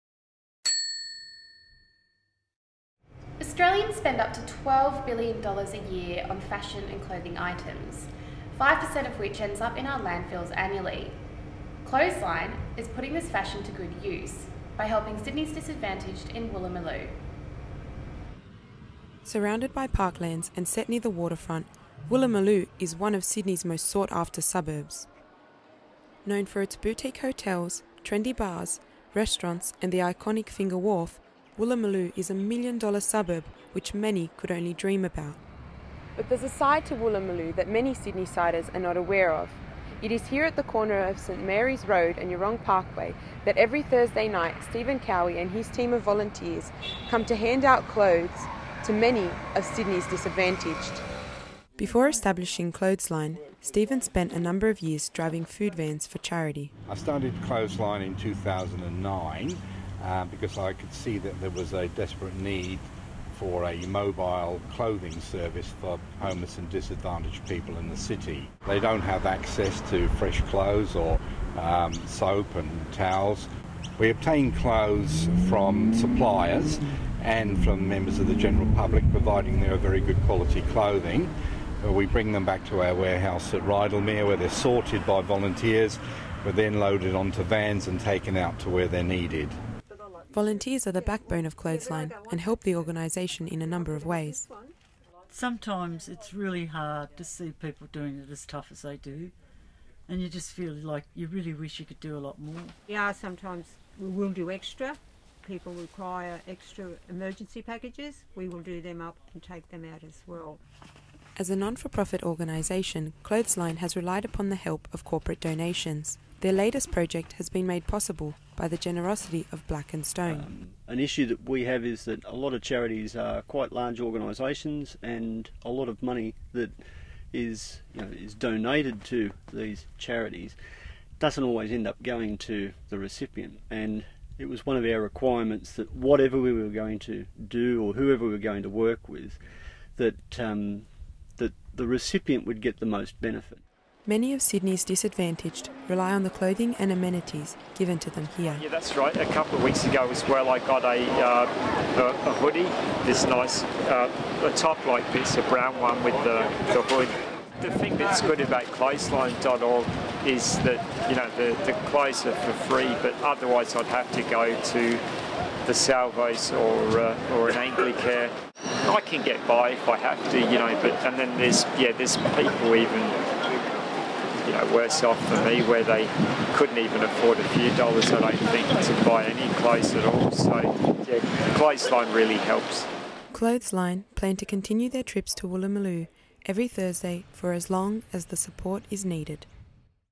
Clothesline
Clothesline is a mobile clothing service for disadvantaged people in the city. Stephen Cowie founded the organisation in 2009 after realising that homeless people in the city are in desperate need of fresh clothes. Clothesline obtains clothes from suppliers and the general public. Volunteers are the backbone of the organisation, sorting through clothes in the warehouse. Chris Florac, from corporate sponsor Black and Stone, explains how, as a sponsor, it is important that donations make it to the hands of the disadvantaged. Finally Paul Bowden, clothing recipient, says that if it wasn't for the convenience of a mobile supplier, he would have to visit a Salvos or Anglicare store.

Created by Angela Ostojic and Sophie Crenigan.